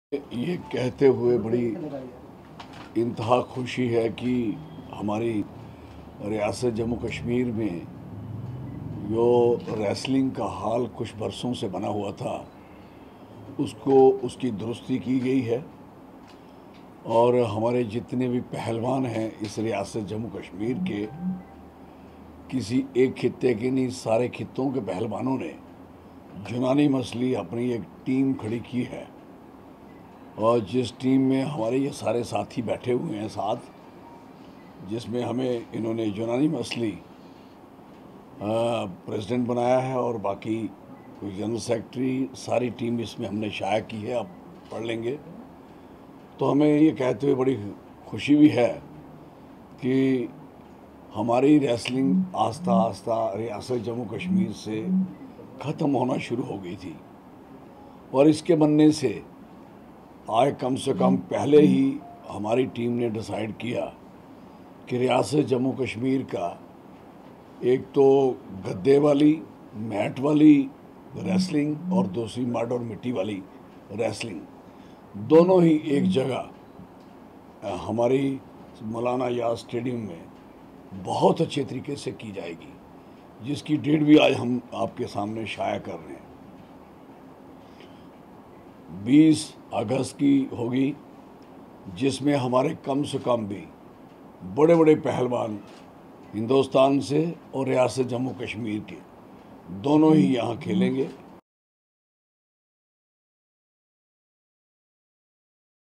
J&K to host national level wrestling championship
| Excelsior News |